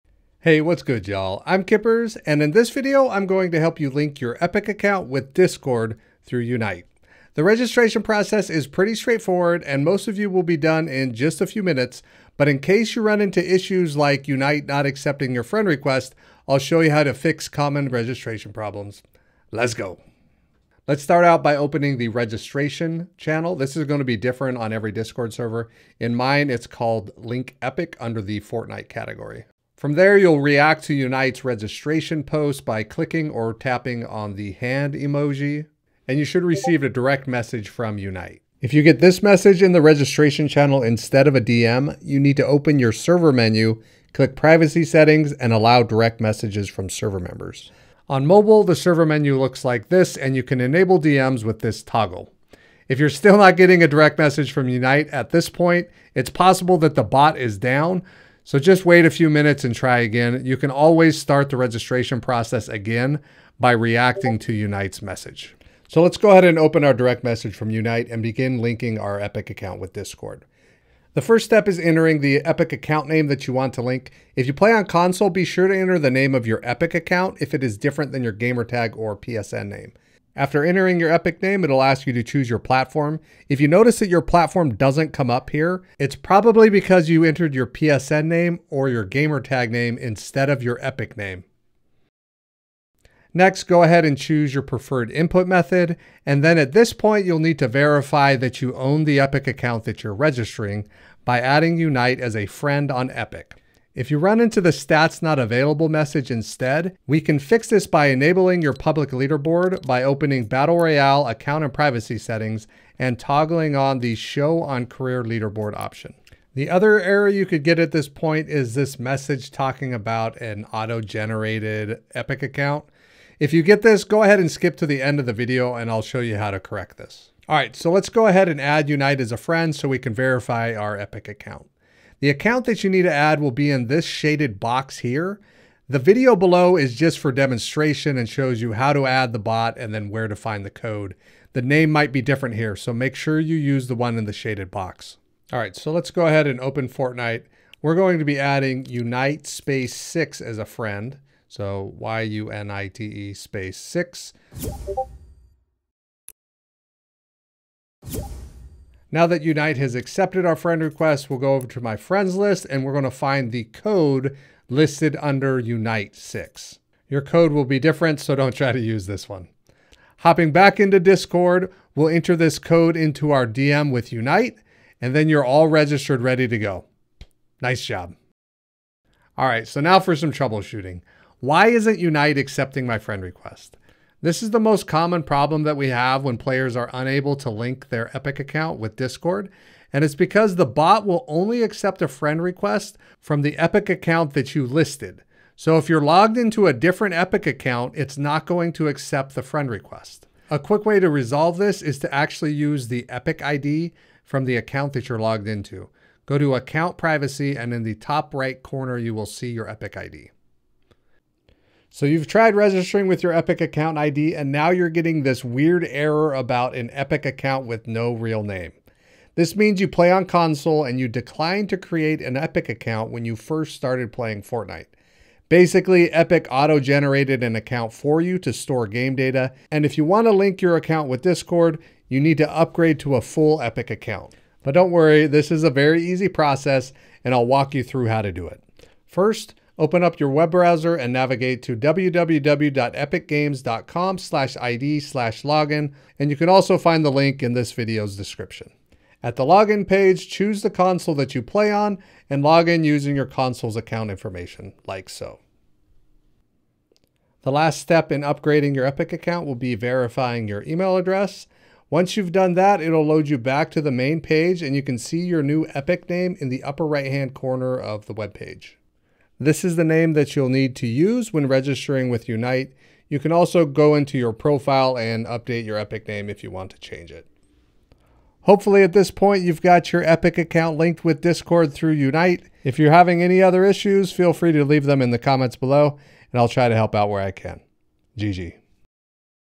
How to Register with Yunite - Linking Epic to Discord Step-by-Step Guide
Why isn't Yunite accepting my friend request? Watch this complete walk-through for verifying your Epic Games and Fortnite account with the Yunite discord bot.